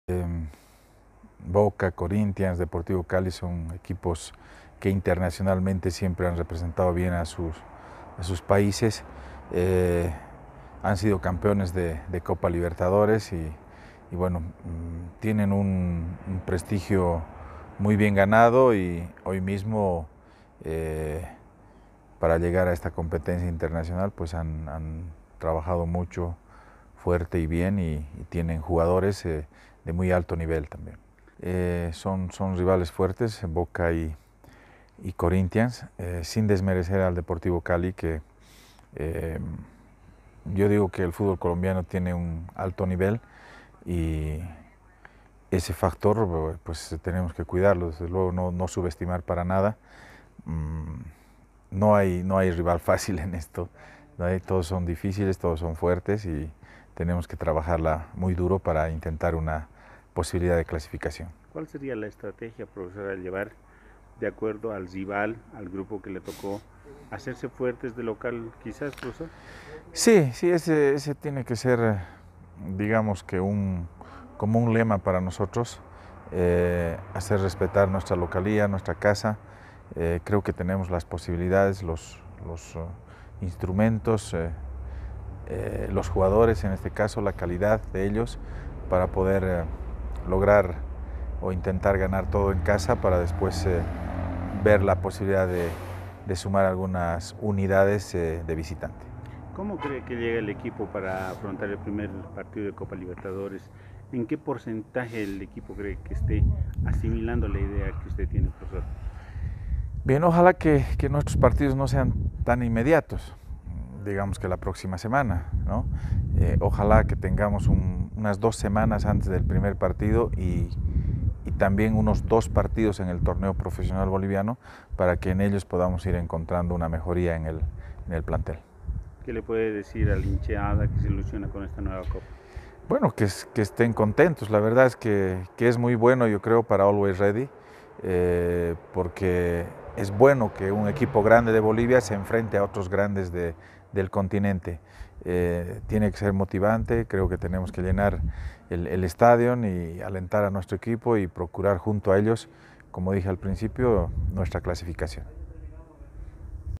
Boca Juniors, Corinthians y Deportivo Cali son los rivales de Always Ready en la Libertadores
El director técnico de Always Ready, Eduardo Villegas, brinda su opinión sobre el grupo que le toco a los alteños en la Copa Libertadores de América 2022.